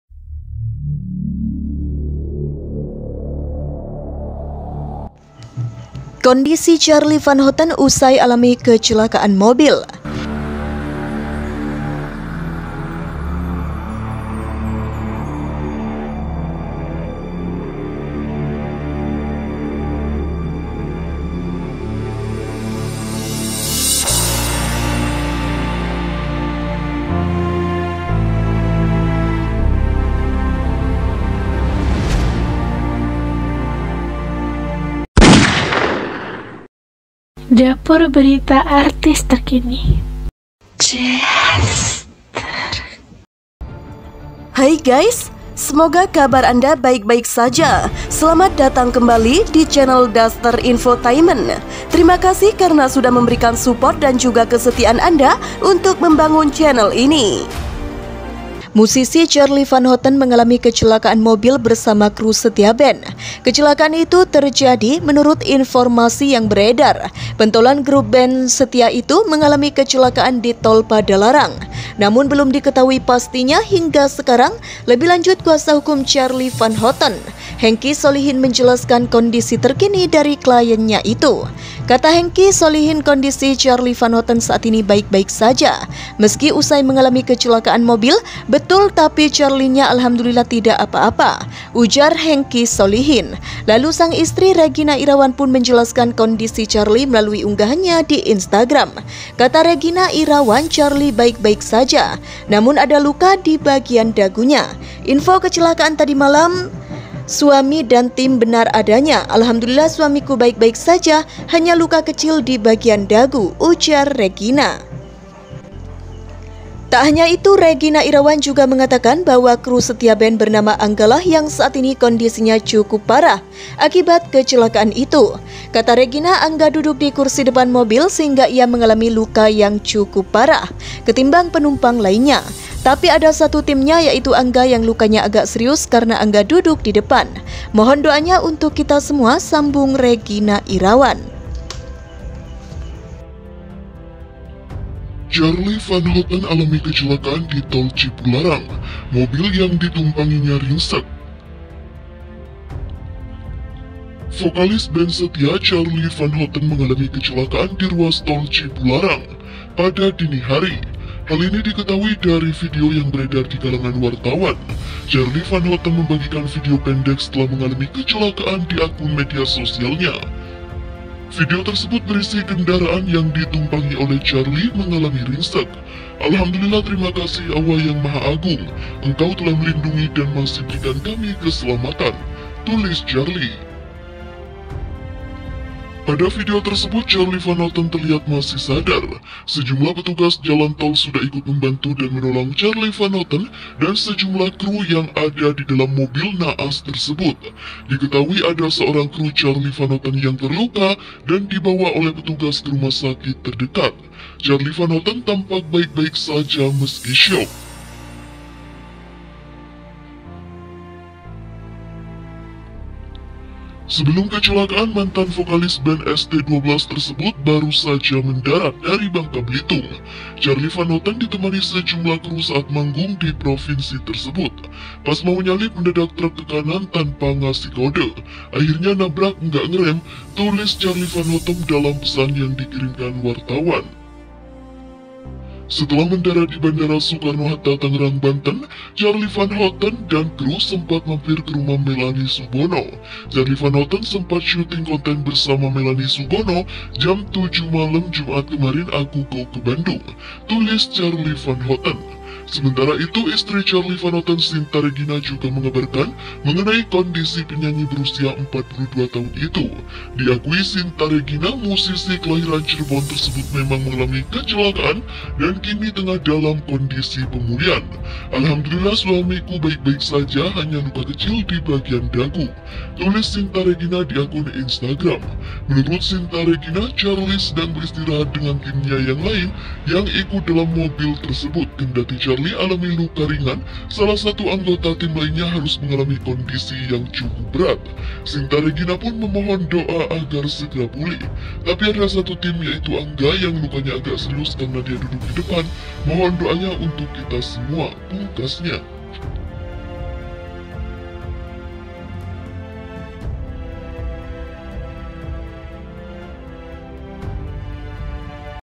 KONDISI TERBARU CHARLY VAN HOUTEN KECELAKAAN DI TOL CIPULARANG DIUNGKAP SOSOK | gosip artis hari ini
KONDISI TERBARU CHARLY VAN HOUTEN KECELAKAAN DI TOL CIPULARANG DIUNGKAP SOSOK INI | gosip artis hari ini

insertlive,
insert today,
insert pagi,
insert siang,
infotainment,
indonesia,